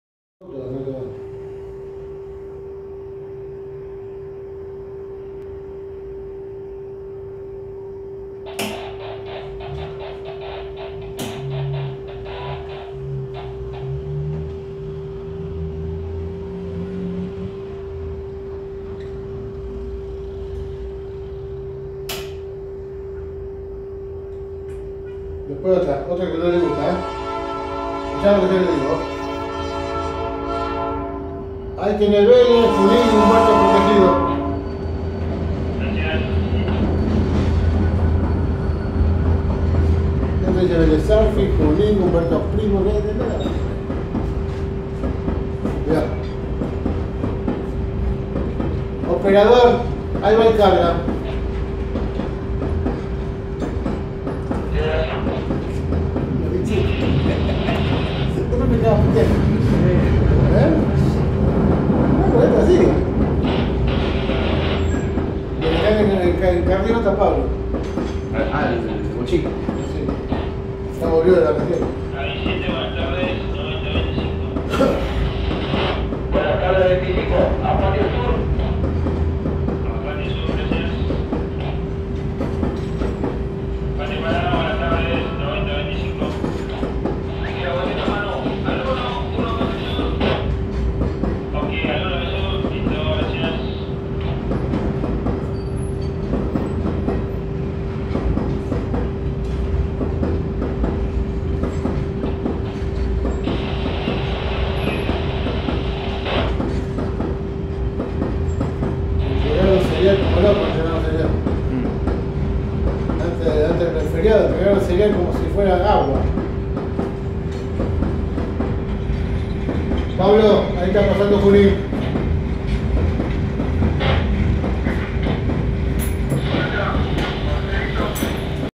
tren vacío de NCA... a patio parada 😁
Jueves 26/05 📅 17:36 ⏰
en la tarde de hoy nublada...
tren vacío de NCA.. procedente de patio San Lorenzo...
hace su pasada por zona norte de rosario..
P.A.N JUAN JOSE PASOS.. 305
rumbo a Patio Parada cabin 7..
dicha formación esta al mando de la loc 🚆GT22 9025 NCA

Rosario - Santa Fe - Argentina 🇦🇷